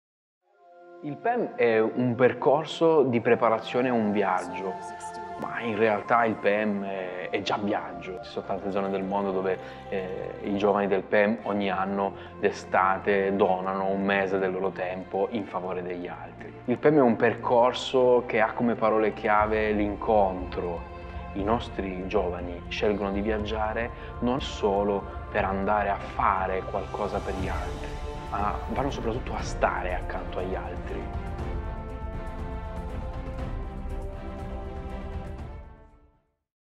Spot PEM - 2019
Spot per il Centro Missionario della Diocesi di Concordia - Pordenone.
Tema: PEM Percorso Esperienza Missionaria
Camera: Giulio Zanier
Montaggio: Giulio Zanier e Matteo Marchetti
Regia: Matteo Marchetti
Foto: Alex Zappalà
Protagonista: Alex Zappalà
Studio: F8 Digital Roveredo in Piano (PN)